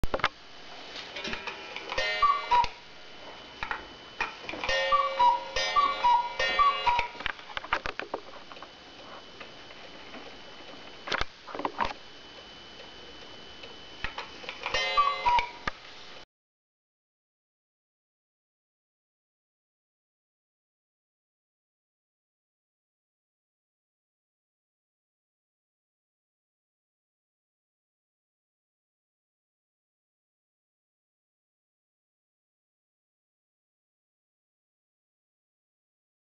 Sternreiter bird and leaf cuckoo clock - 1200T
This charming and inexpensive cuckoo clock has a nice "two-tone" finish with the engraved portions highlighted with a lighter finish. The leaves and bird are hand carved from solid Linden wood. The when the clock strikes the hour or half-hour, the door opens, a cuckoo appears and calls along with the clock's gong strike.

This clock is hand made in Germany and is distributed by Sternreiter and North Coast Imports. The mechanical brass movements is driven by two weights and time escapes with the tick of a traditional pendulum. The cuckoo call sound is made by two wooden whistles with bellows inside the clock case.

Even the packaging has old-world charm, as only paper is used. The factory packing is all made with renewable materials. This clock makes a great gift for the young and old alike.